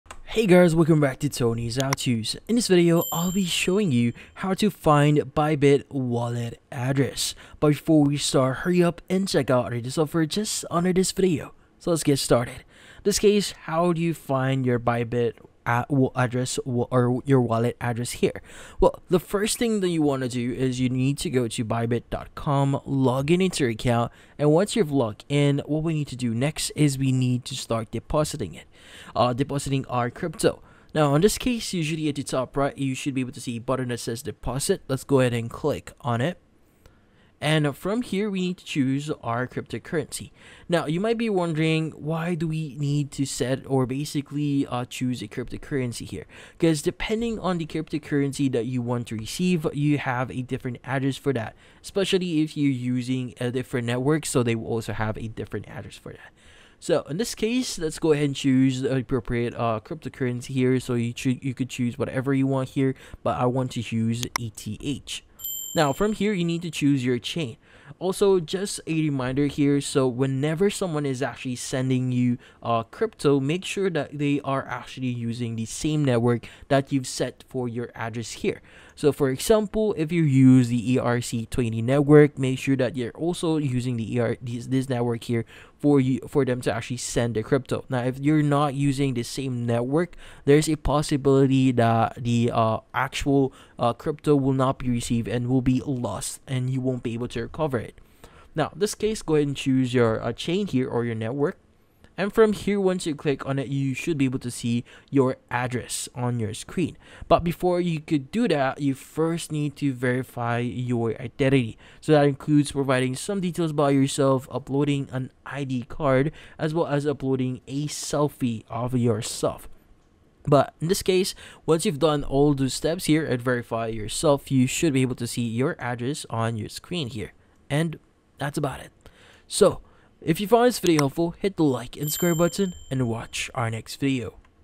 How to Find Bybit Wallet Address (2024)
In this video, I show you How to Find Bybit Wallet Address in 2024. The quick and easy tutorial how to see my bybit wallet address. Do you want to know step by step how to copy usdt wallet address on bybit in 2024? Yes? Great!